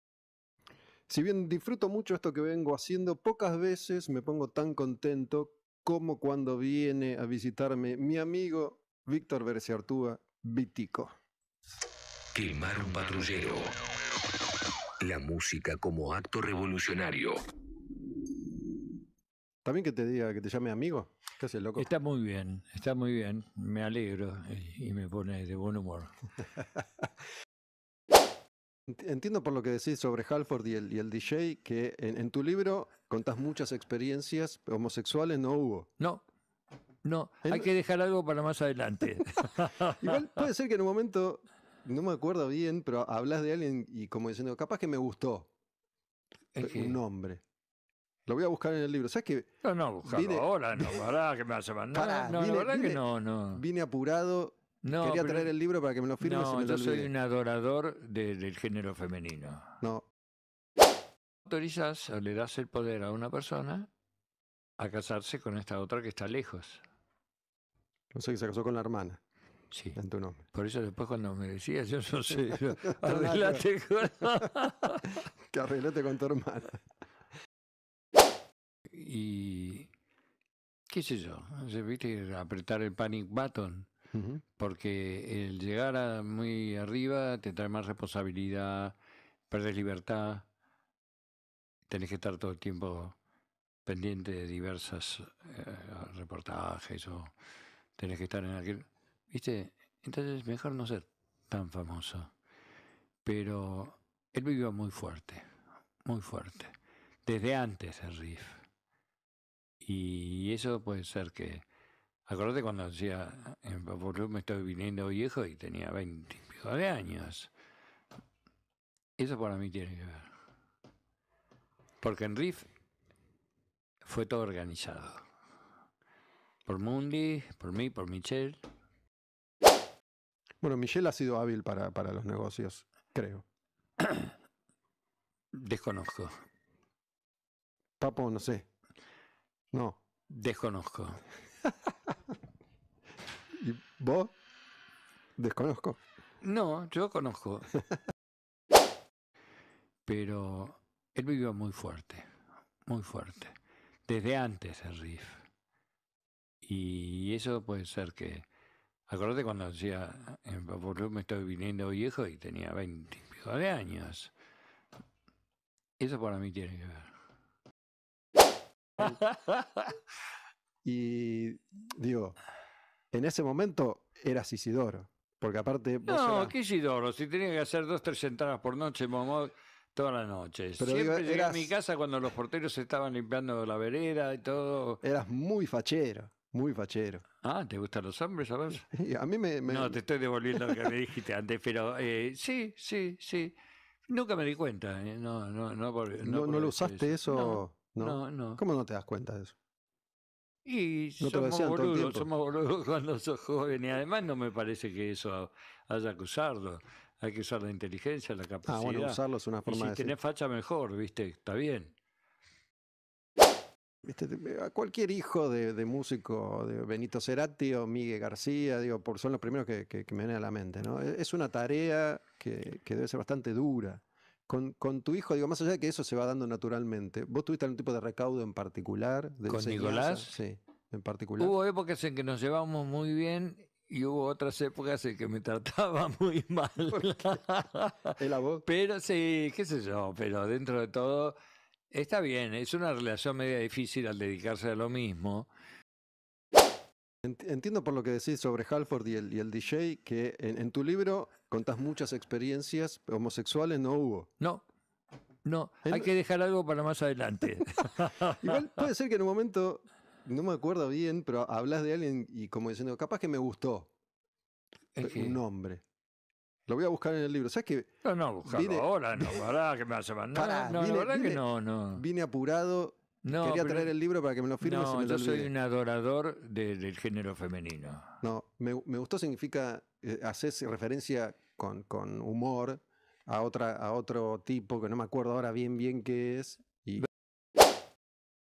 QUEMAR UN PATRULLERO: VITICO
Víctor Bereciartúa, Vitico, es un músico argentino con una historia a la altura de cualquier estrella de rock internacional. Con la edición de su libro El Canciller como excusa nos juntamos nuevamente para charlar y reír con seriedad. Al lado de Pappo se hizo enorme como bajista de Riff. Existe una expresión en inglés que lo describe a la perfección: Larger Than Life! Tiene más de 70 años y aún le queda mucho por hacer... Je.